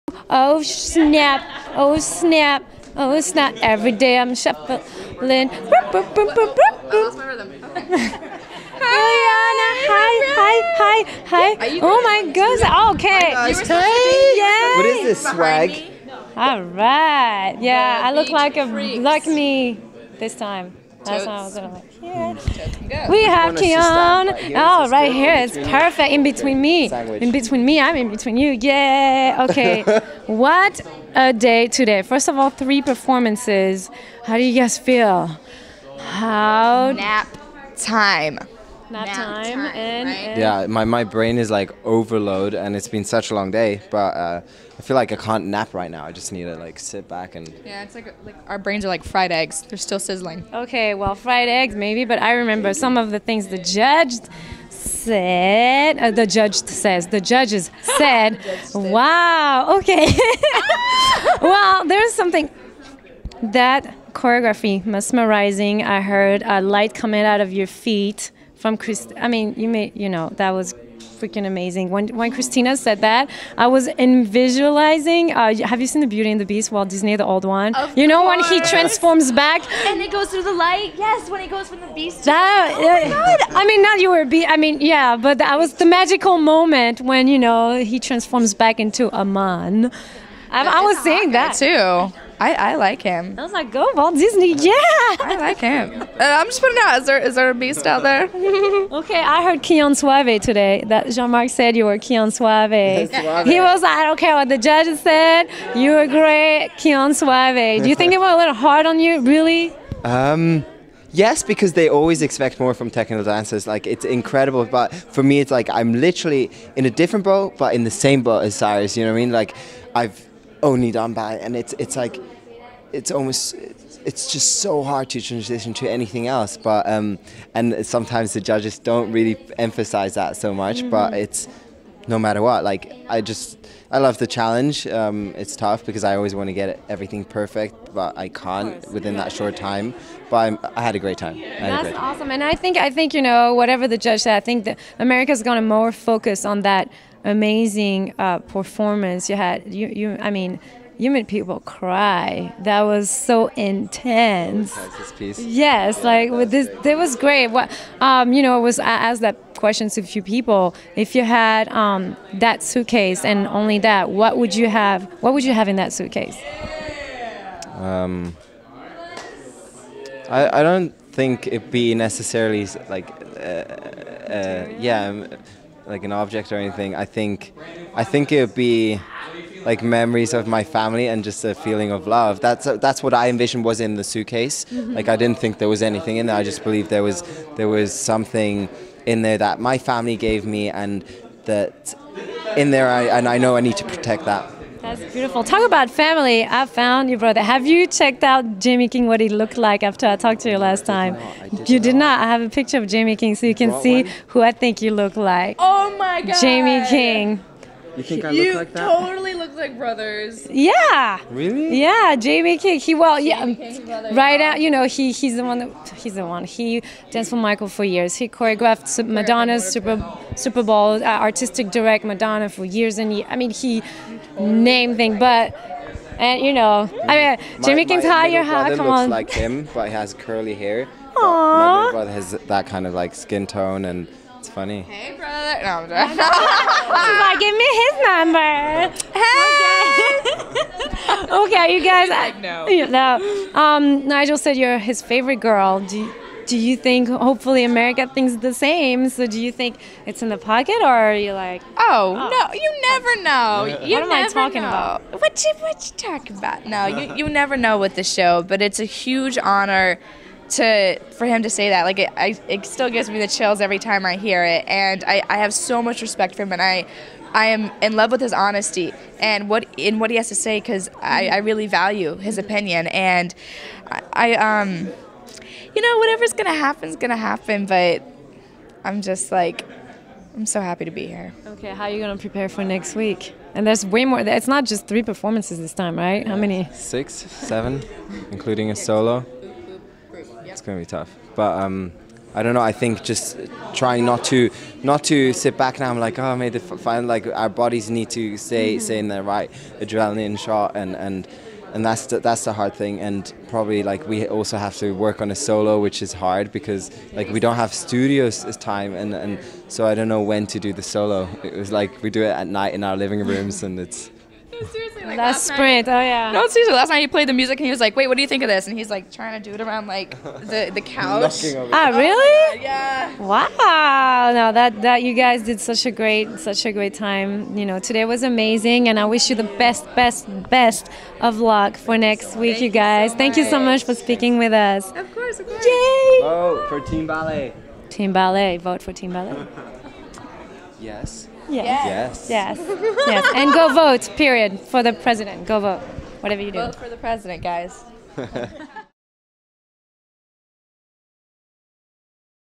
Chehon and Eliana talk to Maryss From Paris about Top 4 SYTYCD
Pacific Rim Video / Front Row Features Wire celebrity guest correspondent Maryss From Paris of The Beat Freaks talks to Top 4 Finalists Chehon Wespi-Tschopp and Eliana Girard on making it to the finals, Chehon looking like Jamie King and more after the live taping of So You Think You Can Dance Season 9 at the CBS Studios Los Angeles on Wednesday, September 5, 2012. Video produced by Peter Gonzaga.